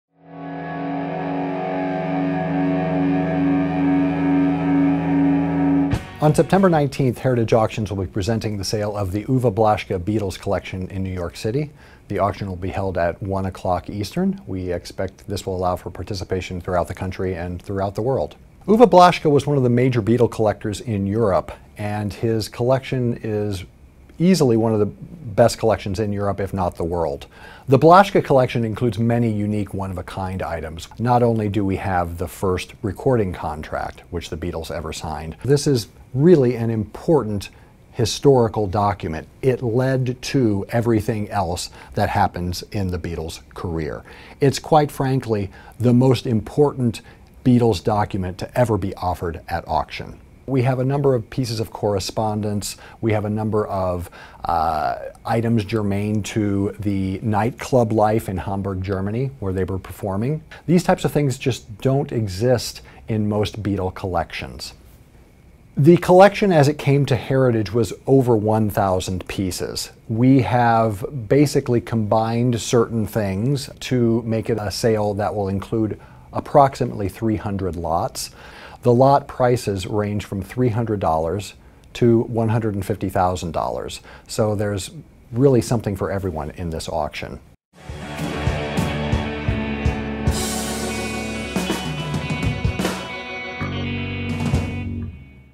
The Uwe Blaschke Beatles Collection
Heritage Auctions (HA.com) -- The Uwe Blaschke Beatles Collection is being offered for the first time at auction on September 19 - 20, 2015.